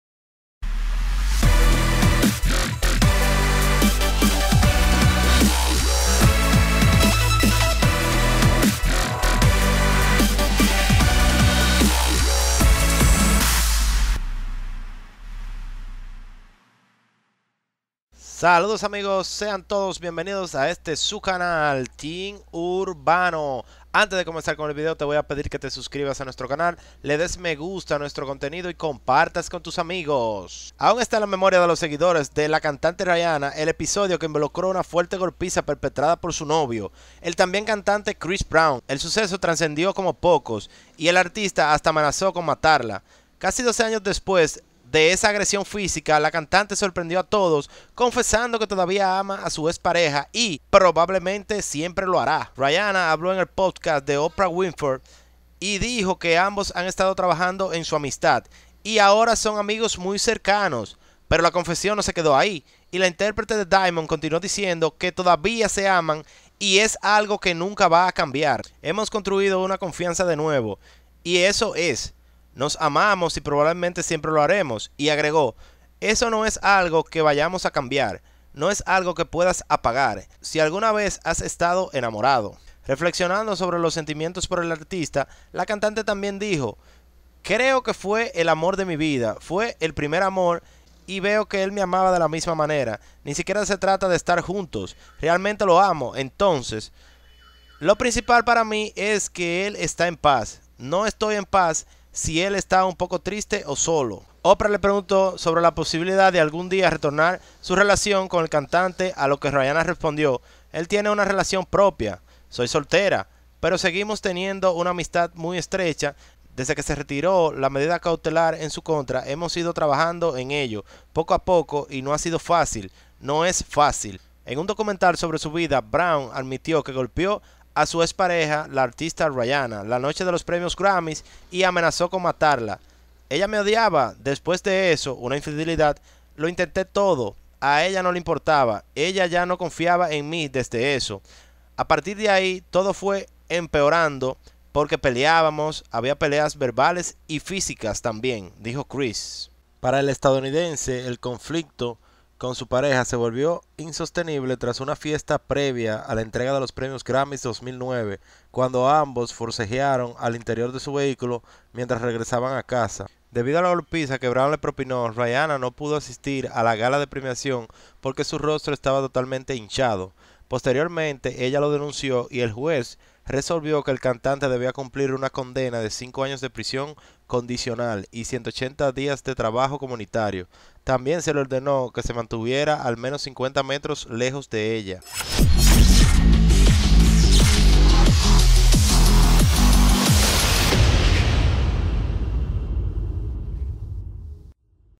A pesar de la golpiza, Rihanna confiesa que todavía ama a Chris Brown
A pesar de la golpiza, Rihanna confiesa que todavía ama a ChrisBrown